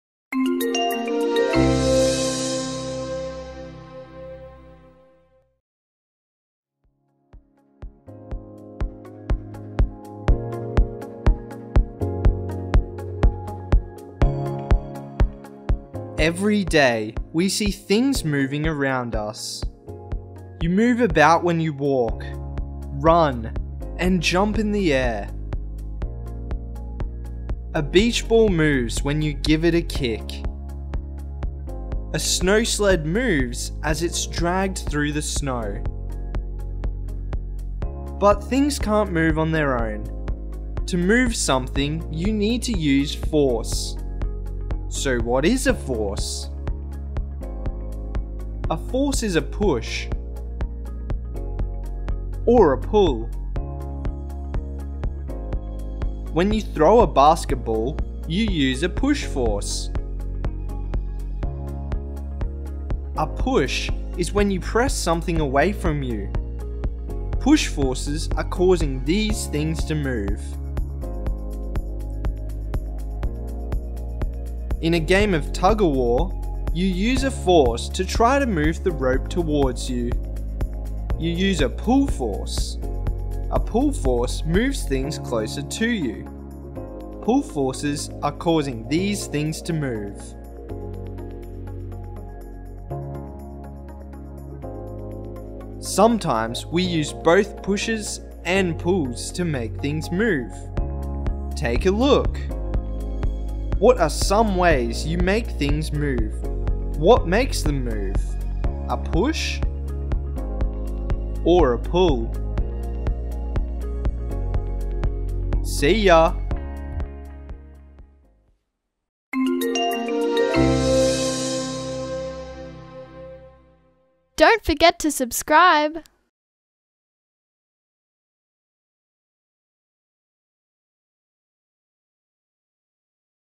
What is a Force?
What are forces? How do we use forces from day to day?

Everyday, we see things moving around us.
You move about when you walk, run and jump in the air.
A beachball moves when you give it a kick. A snow sled moves as its dragged through the snow.  But things can't move on their own. To move something you need to use force. So what is a force?

A force is a push or a pull.

When you throw a basketball, you use a push force. A push is when you press something way from you. Push forces are cauing these things to move.

In a game of tug of war you, you use force to try to move the rope toward you. You use a pull force. A pull force moves things closer to you. Pull forces are causing these things to move.

Sometimes we use both pushes and pulls to make things move. Take a look.

What are some ways you make things move? What makes them move – a push or a pull?